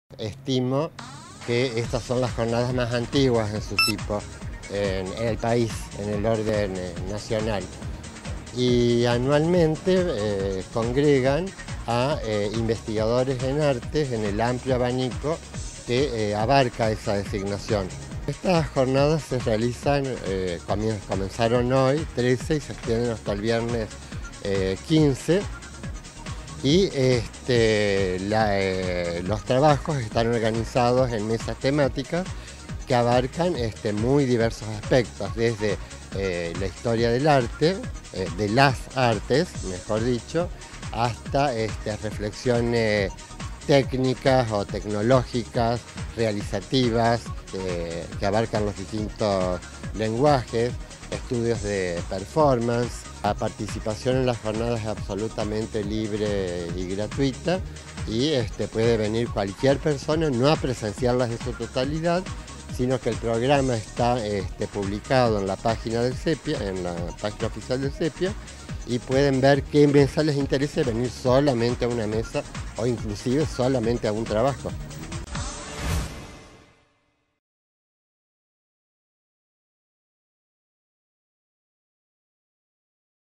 XXI Jornadas de Investigación en Artes - 15.09.17
Finaliza hoy una nueva edición de las jornadas anuales que congregan a  diferentes universidades y centros de investigación del país en un espacio integrador de diferentes disciplinas artísticas